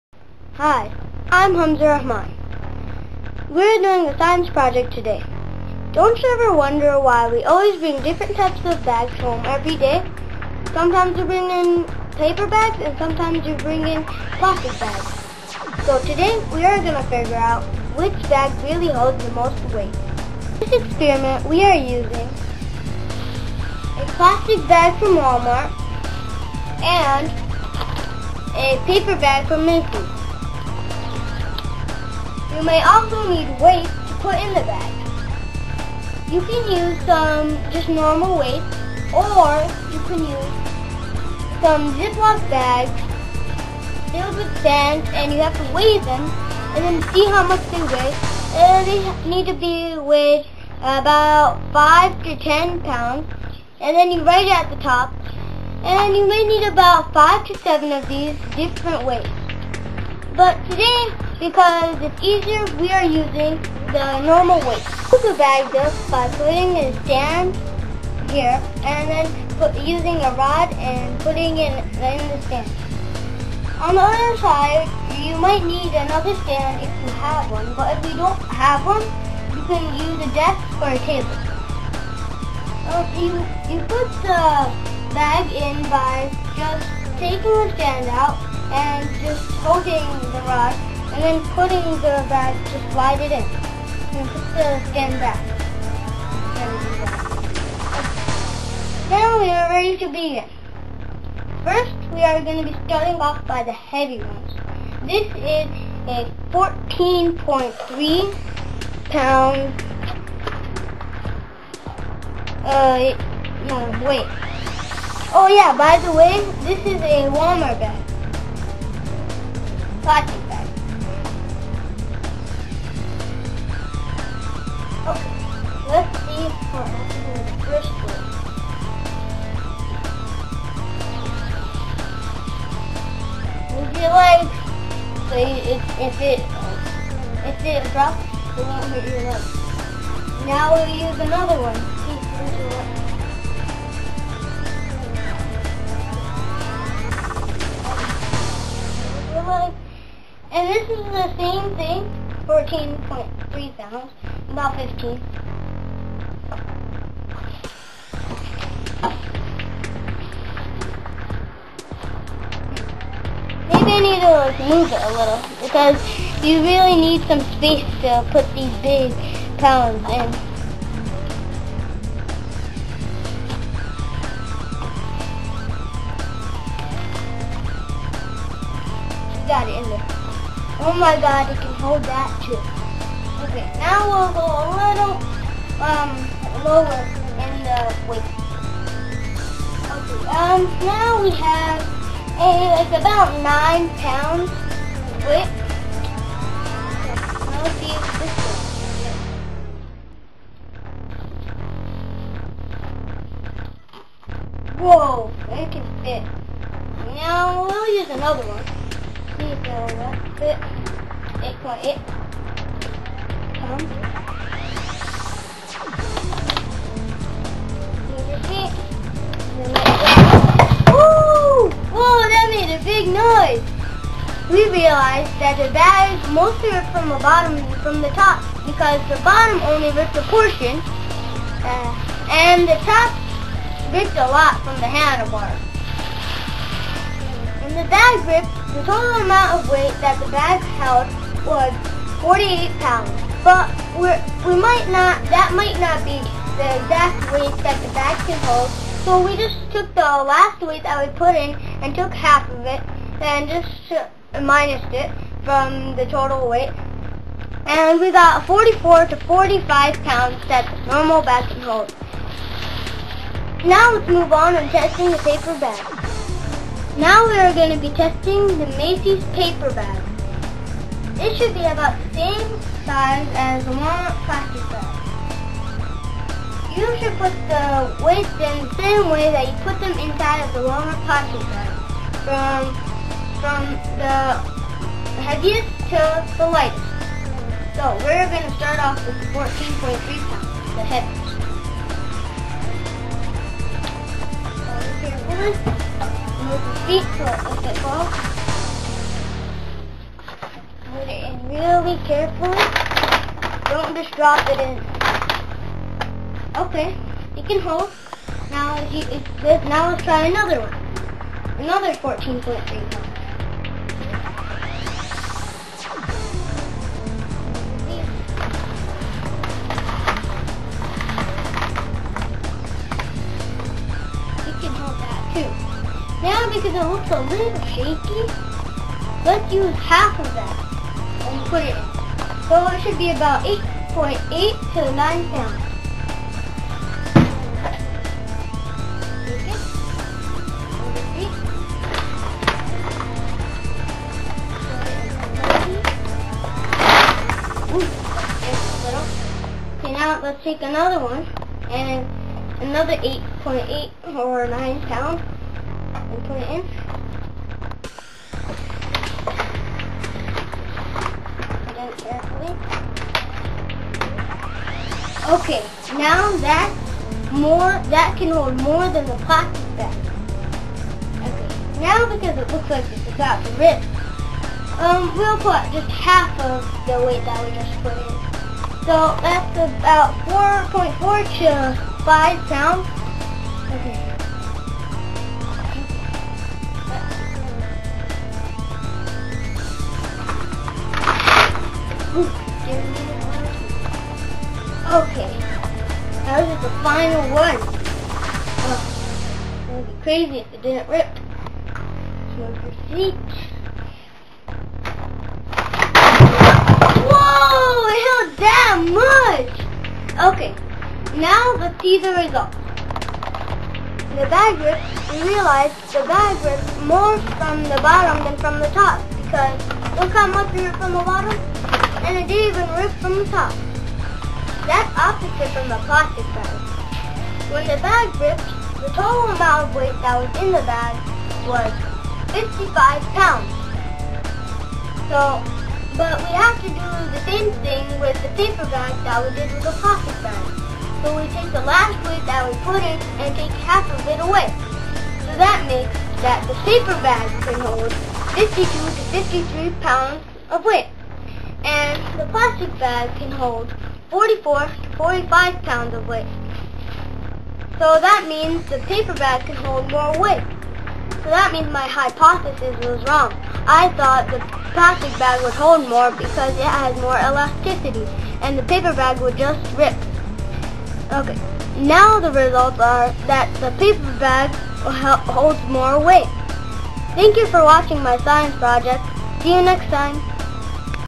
Shopping Bag Strength - Science Project
eHamza.com presents 'Shopping Bag Strength'

Objectives: To determine the strength of a particular type of bag (for example, plastic bags) and exactly how much the strength increases when multiple bags are used.
Concept: Take a plastic bag and fill it with stuff until it breaks.

Hypothesis:
Which pattern of growth of strength do you think you will see as more bags are added? Think carefully about the concept questions and then formulate your hypothesis.